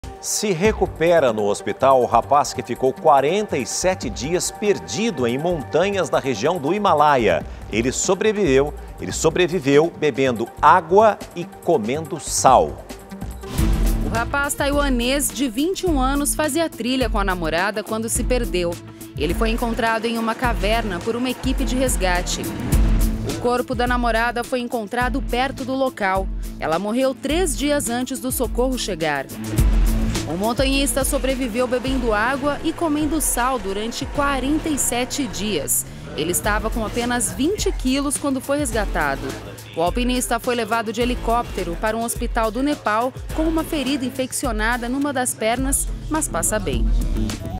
Rapaz perdido 47 dias em trilha se recupera em hospital
O montanhista taiwanês ficou 47 dias perdido na região do Himalaia. Ele sobreviveu bebendo água e comendo sal e foi resgatado com apenas 20 kg. A namorada, que iniciou a trilha com ele, foi encontrada morta.